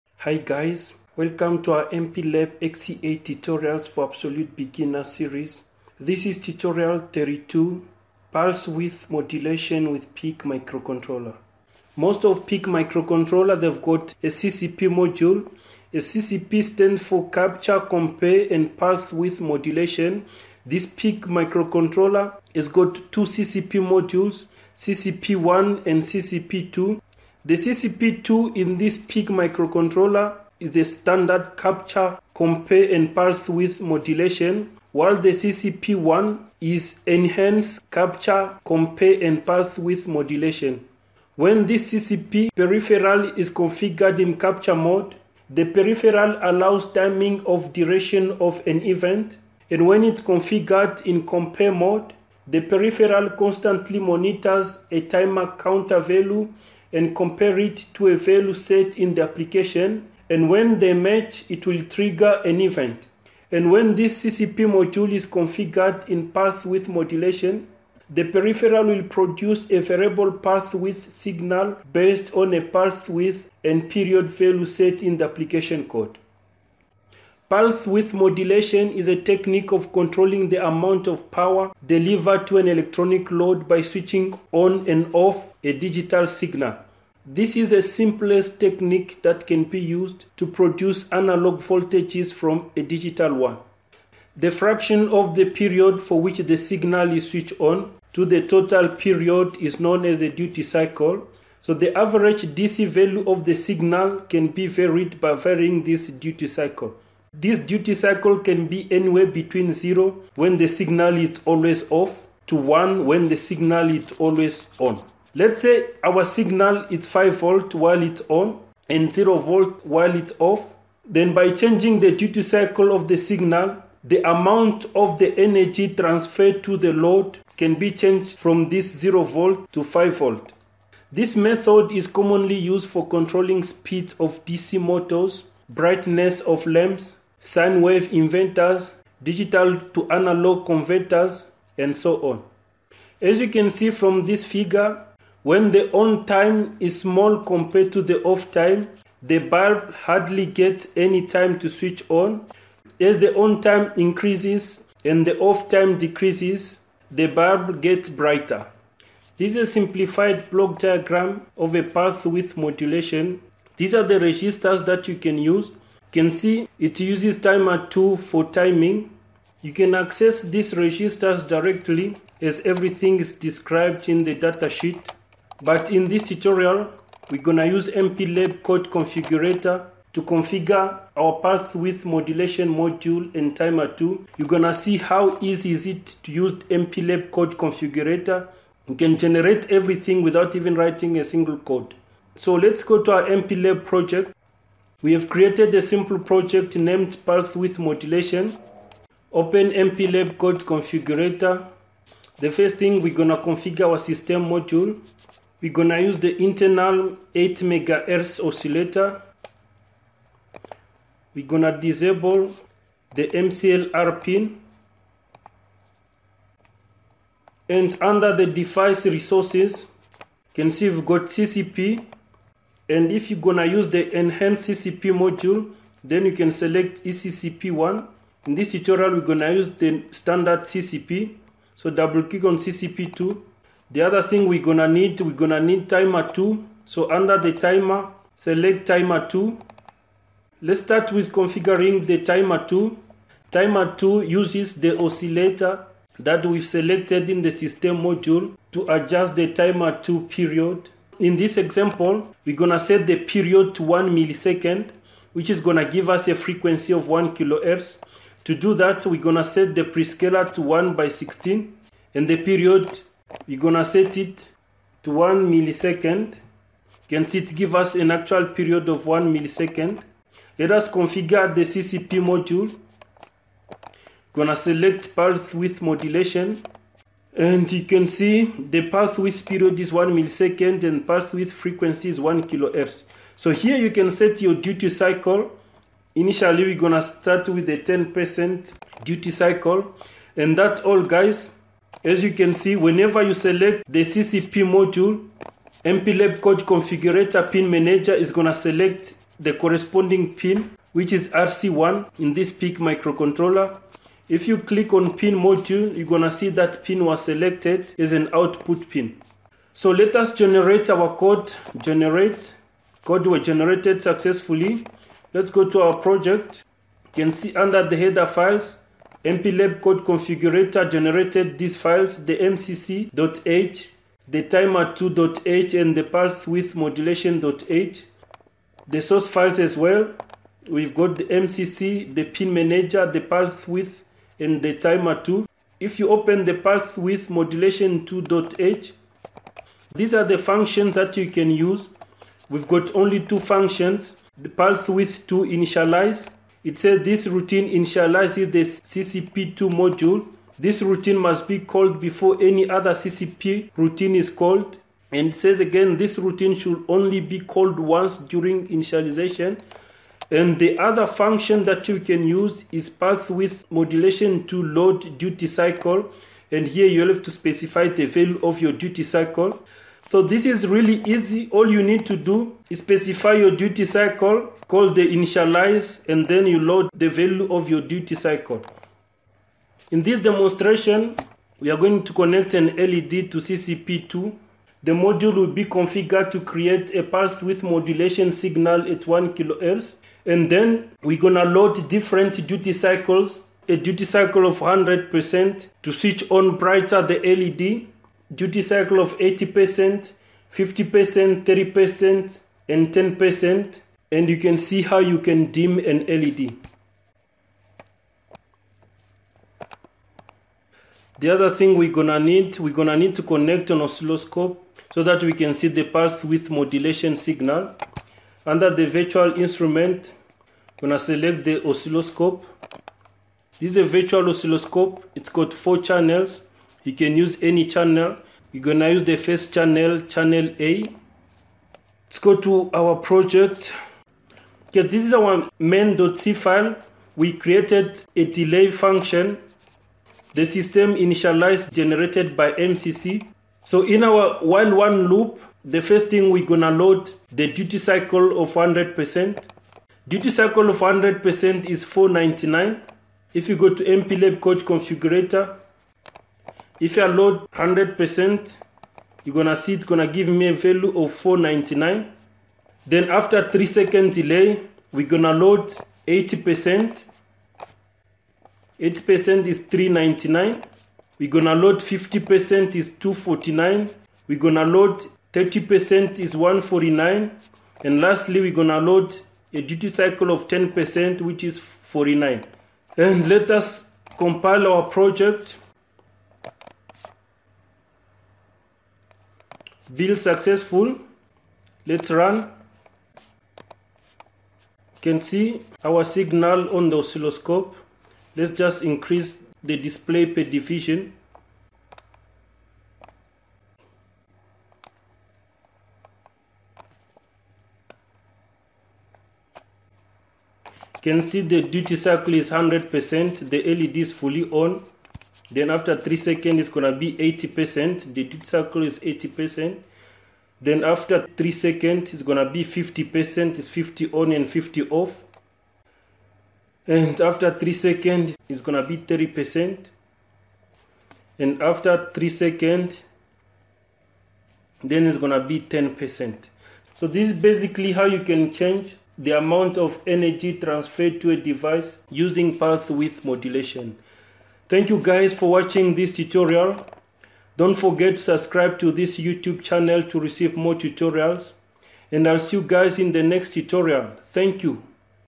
32- Pulse Width Modulation (PWM) | MPLAB XC8 for Beginners Tutorial
PWM is a technique of controlling an amount of power delivered to a load by switching ON and OFF a digital signal. Learn to configure PWM, capture and compare with MPLAB Code Configurator.
Have questions, need assistance or looking for source code?